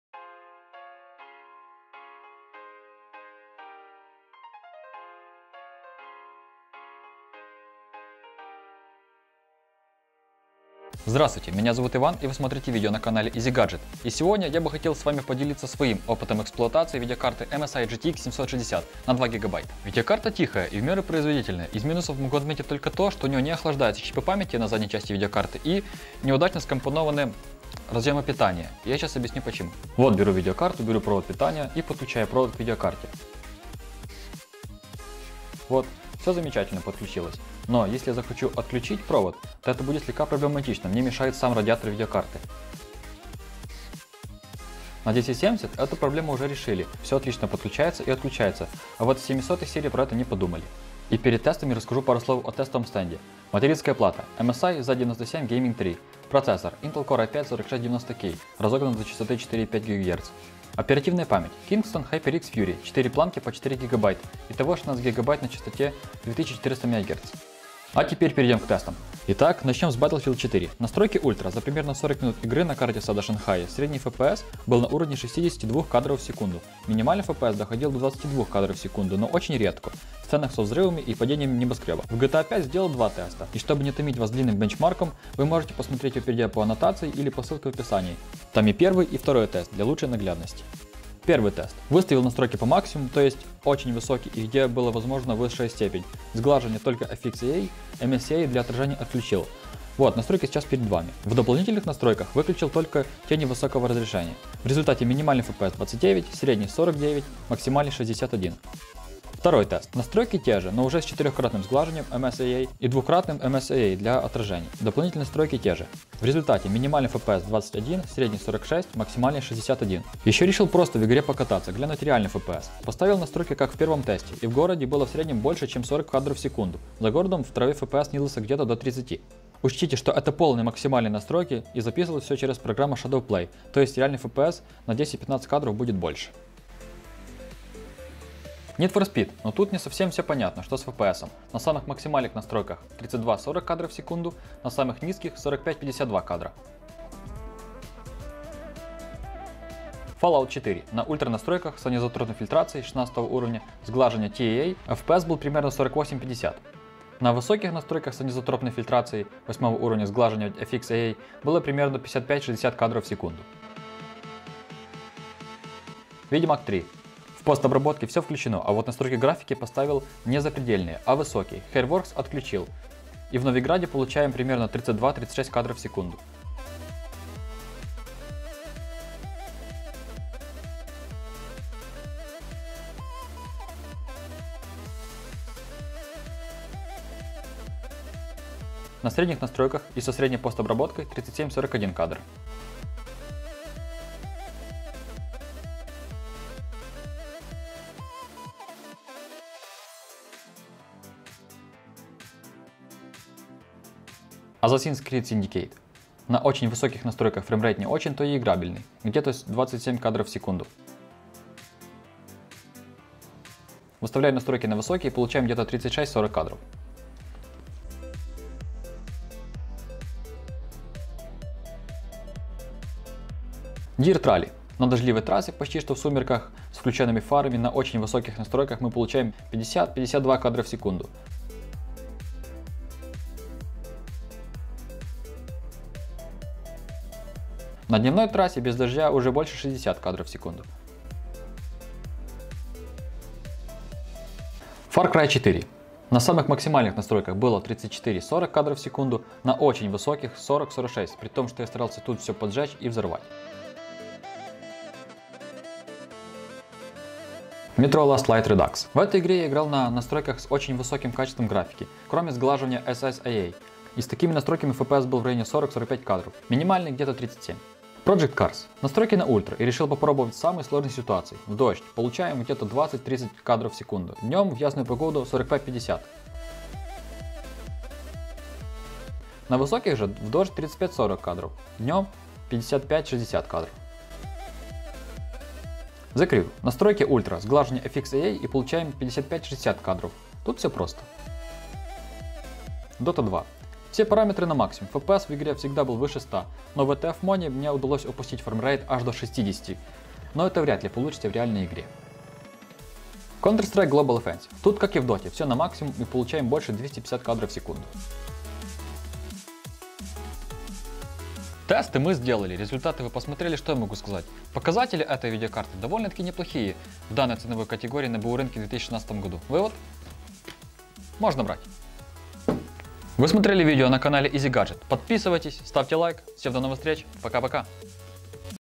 MSI GeForce GTX 760 2GB, Обзор и тестирование, Опыт эксплуатации
Хотел поделиться с вами моим опытом эксплуатации видеокартой GTX760 на 2 гигабайта от компании MSI.